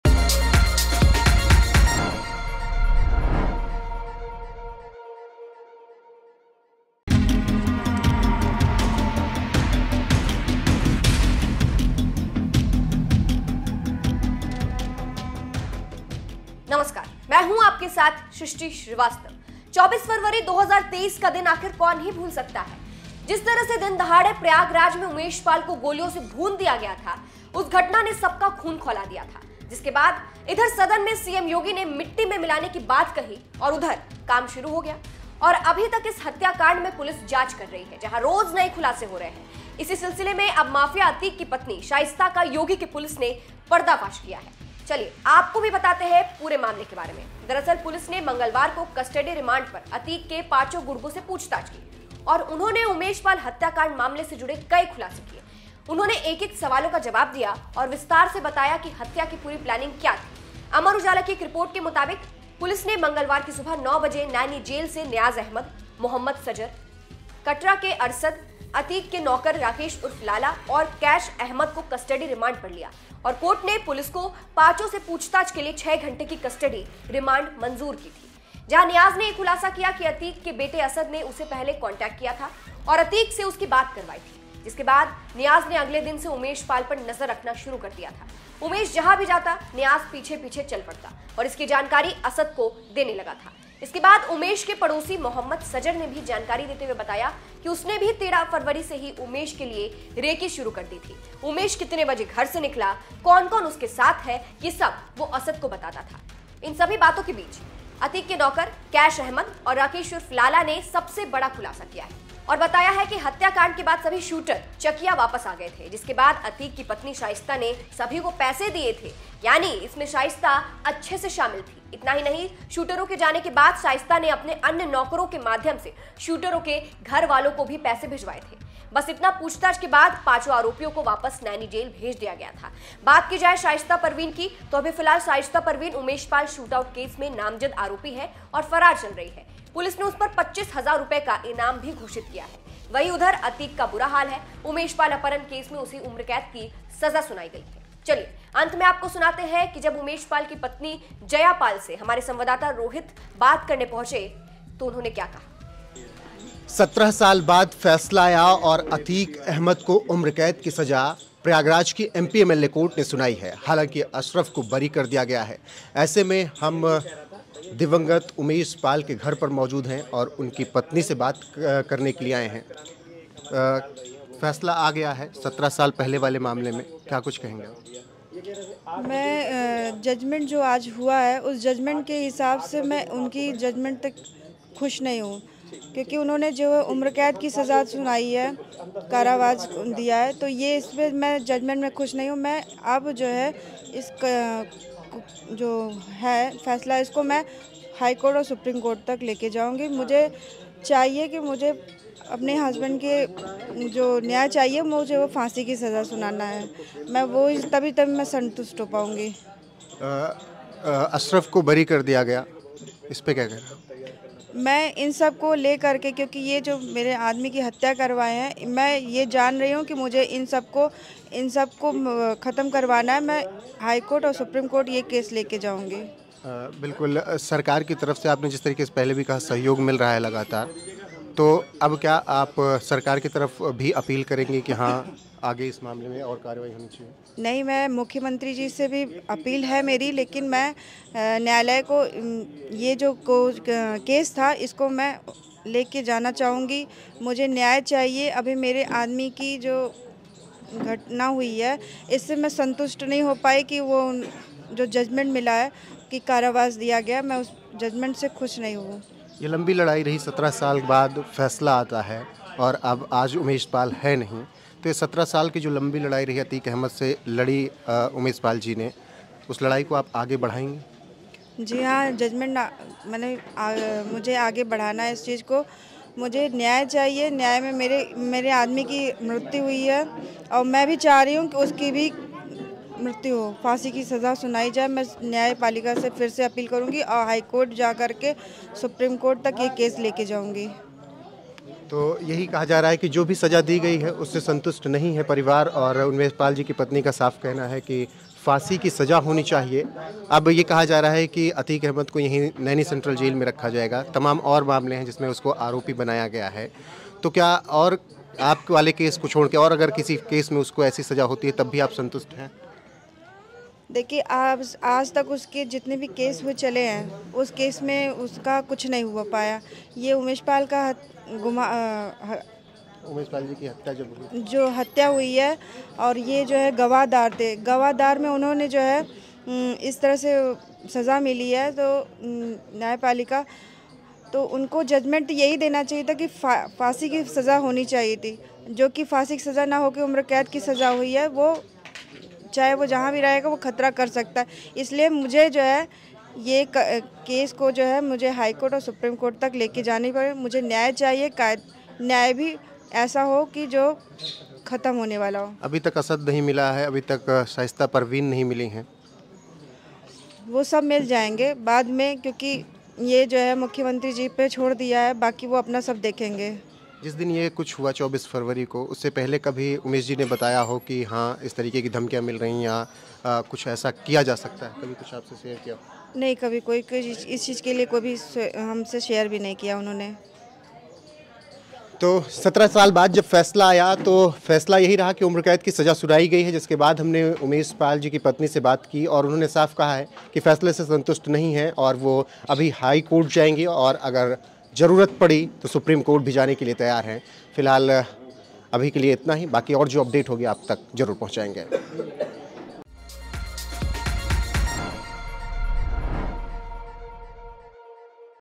Yogi की पुलिस ने Atique की पत्नी शाइस्ता का किया भंडाफोड़, हो गया बड़ा खुलासा
24 फरवरी 2023 का दिन आखिर कौन ही भूल सकता है, जिस तरह से दिनदहाड़े प्रयागराज में उमेश पाल को गोलियों से भून दिया गया था उस घटना ने सबका खून खौला दिया था। जिसके बाद इधर सदन में सीएम योगी ने मिट्टी में मिलाने की बात कही और उधर काम शुरू हो गया, और अभी तक इस हत्याकांड में पुलिस जांच कर रही है जहा रोज़ नए खुलासे भी हो रहे है। इसी सिलसिले में अब माफिया अतीक की पत्नी शाइस्ता का पुलिस ने पर्दाफाश किया है।

Producer- Namrata